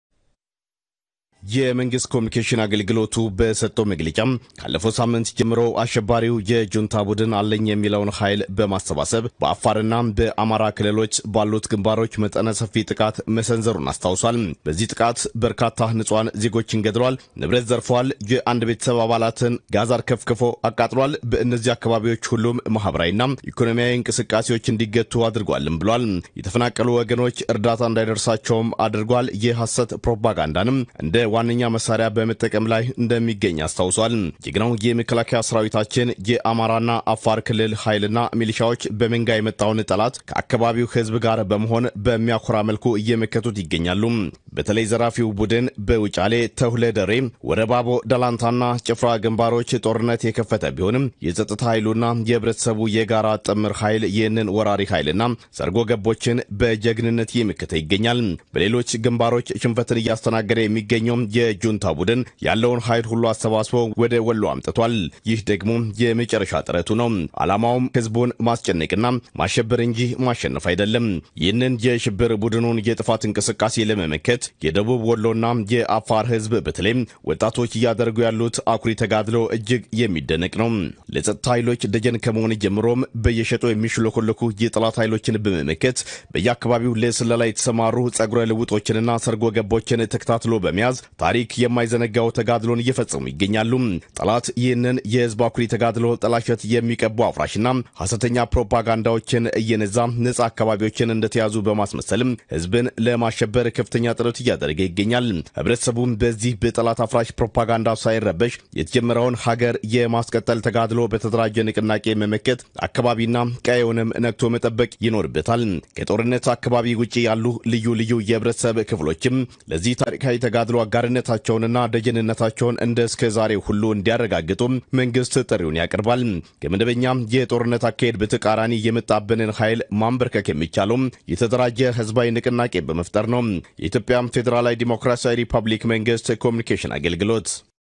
የተከፈተብንን ጥቃት ለመመከት መፍትሄው የተደራጀ ንቅናቄ መሆኑን የመንግስት ኮሚዩኒኬሽን አገልግሎት አስታወቀ
ዘጋቢ፡ ጸጋአብ ዮሃንስ